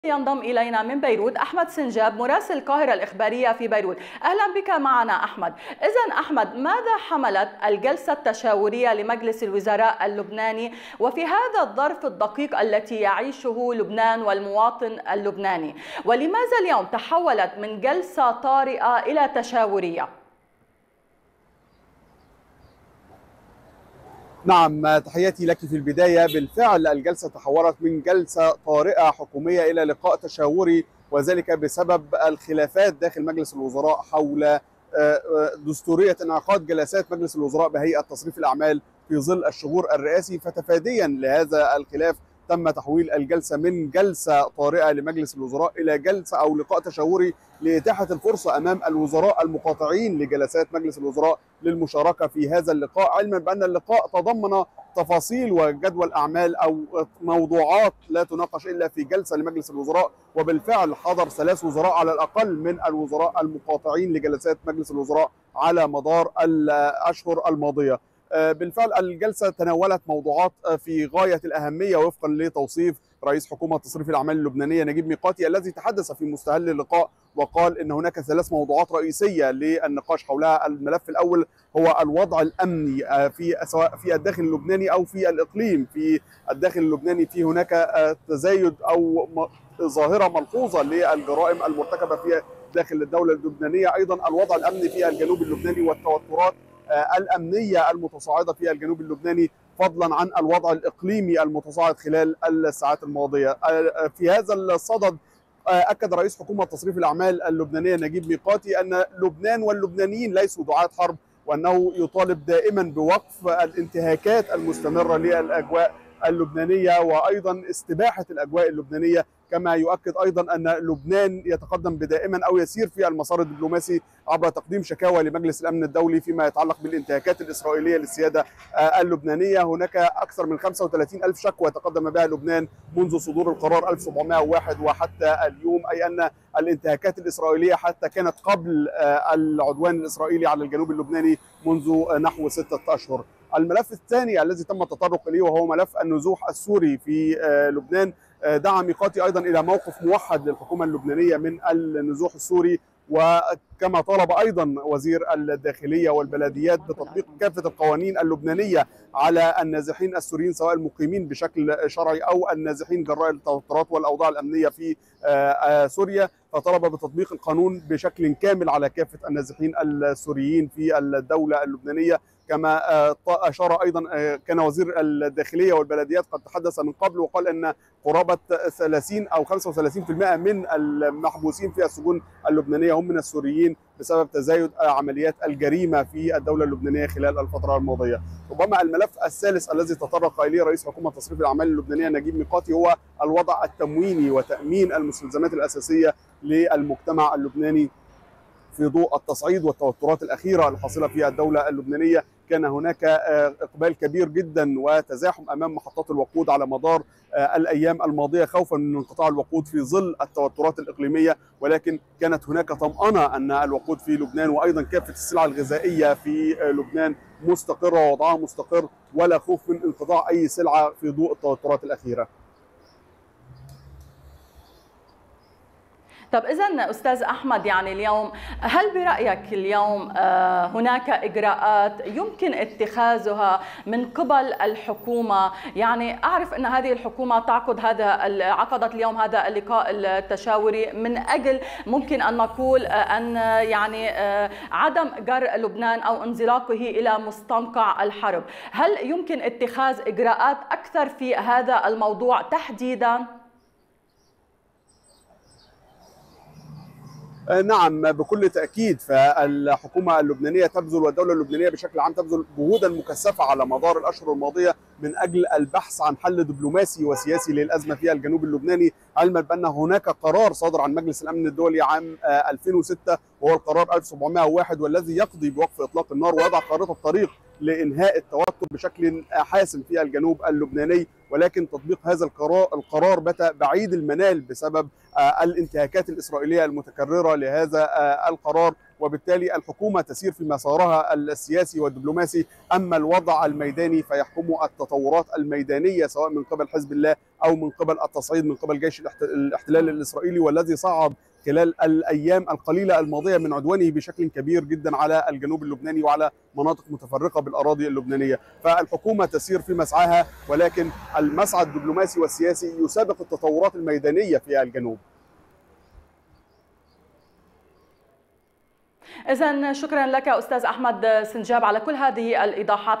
الخلافات السبب.. ميقاتي يستبدل الجلسة الحكومية الطارئة بجلسة تشاورية| التفاصيل الكاملة مع مراسلنا
15/4/2024